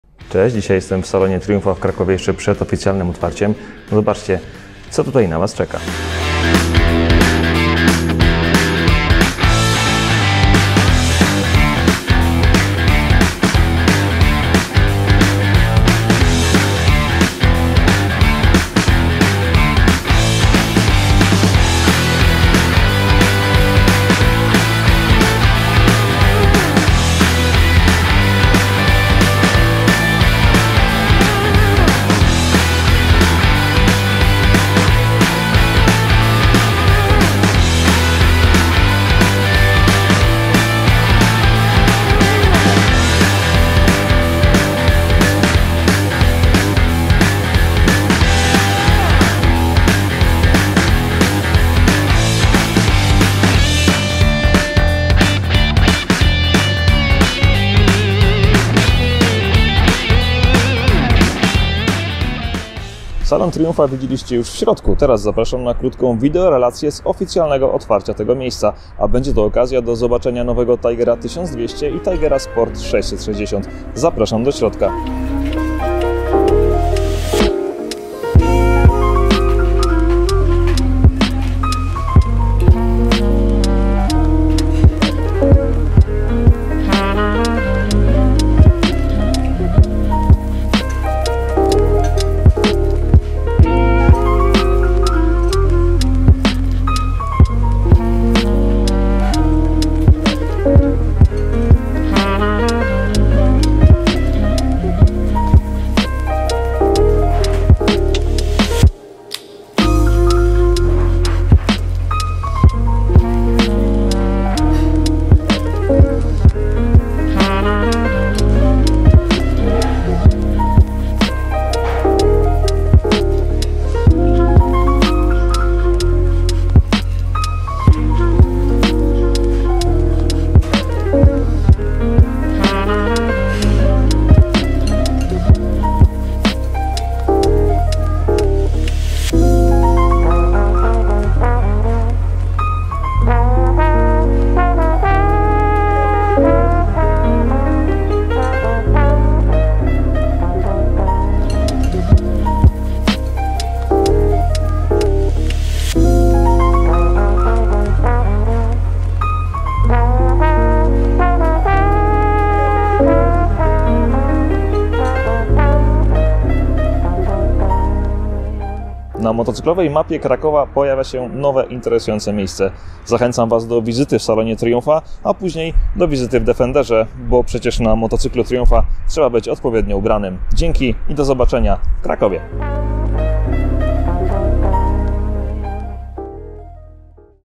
Motocykle Triumph | Kraków - Nowy Salon | Tiger 1200 i Tiger Sport 660
W Krakowie otwarto nowy salon motocyklowy marki Triumph, jest on tym samym szóstym salonem tej marki w Polsce. Miejsce, które koniecznie powinien odwiedzić każdy miłośnik jednośladów. Piękne motocykle dopracowane w najmniejszym detalu oraz spora ilość maszyn na miejscu powodują, że można tutaj spędzić wiele godzin. Bardzo się cieszę, że w Krakowie jest takie miejsce i to jeszcze w tak wysokim standardzie. Uczestniczyłem w oficjalnym otwarciu tego miejsca. Podczas tej imprezy zostały zaprezentowane nowości na 2022 rok. Triumph Tiger 1200 oraz Triumph Tiger Sport 660. Zapraszam na videorelację. Wpadnijcie koniecznie do salonu Triumpha. Czeka profesjonalna obsługa, dobra kawa i fantastyczne motocykle.

🔥 Komentuj, a jeśli masz pytania - to również z nimi śmiało!
📢 Udostępnij ten film - będzie mi niezmiernie miło :)